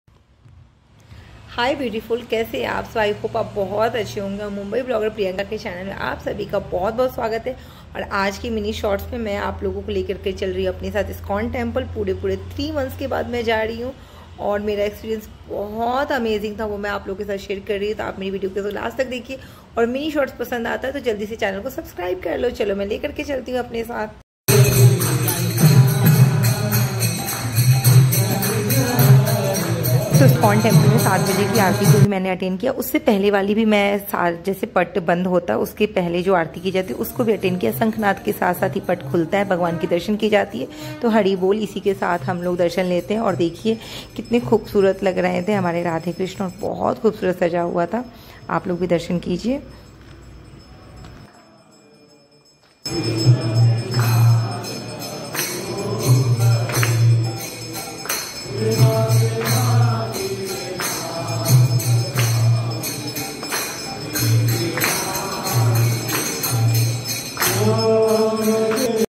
Radhekrishnaदोस्तों आज आप सबke साथ Iskon Temple🛕aarti shareकर रहे हैंl
Radhekrishna दोस्तों आज आप सब ke साथ Iskon Temple 🛕 aarti share कर रहे है.
Mumbai vlogger Priyanka...